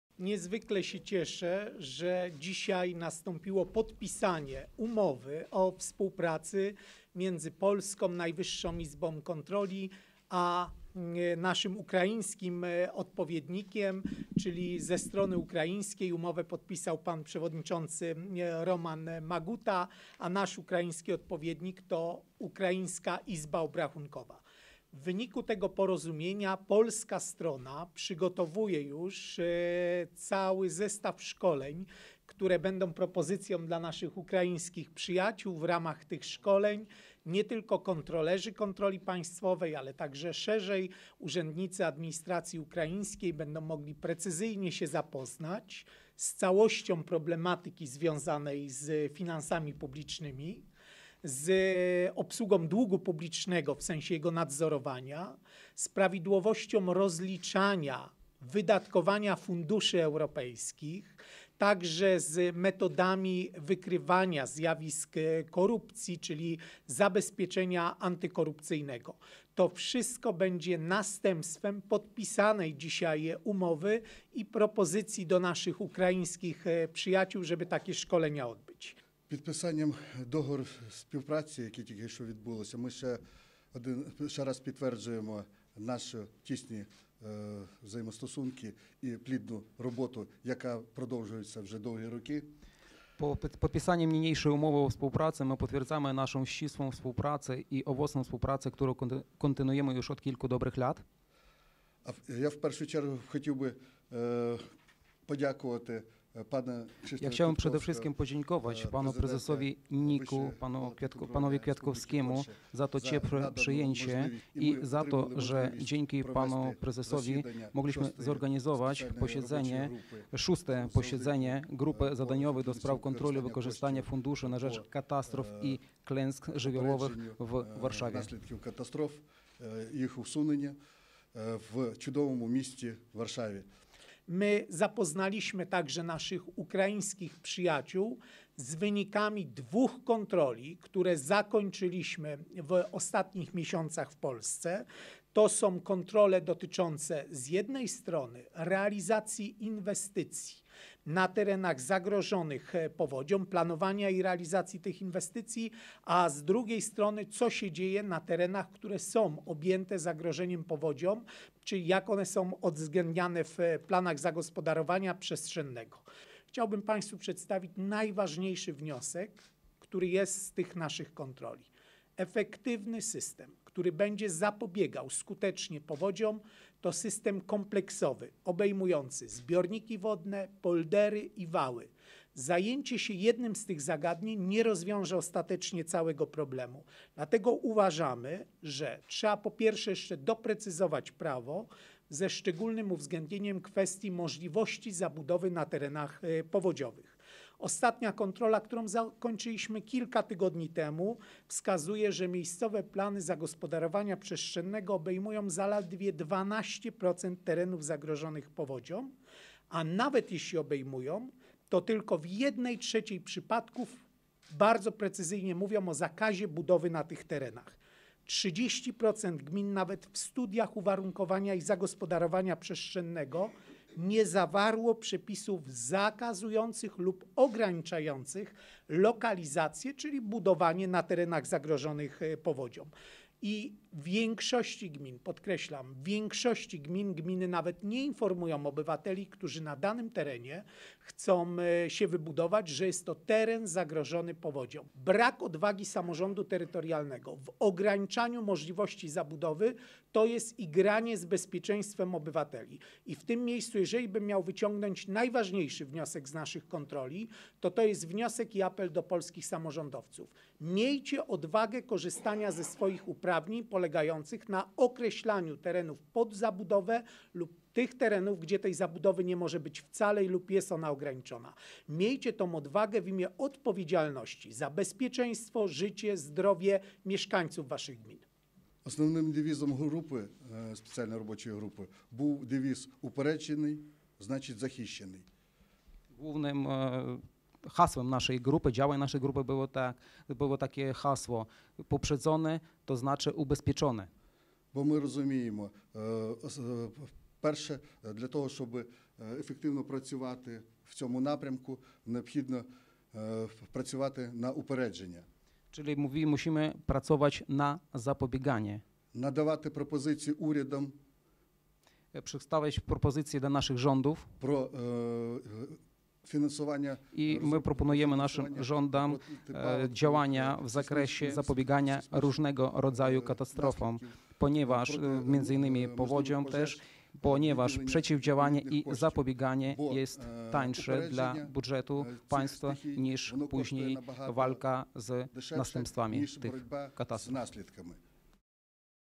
Polska i Ukraina - kontrole dotyczące klęsk żywiołowych
Porozumienie polskich i ukraińskich kontrolerów. Skrót konferencji prasowej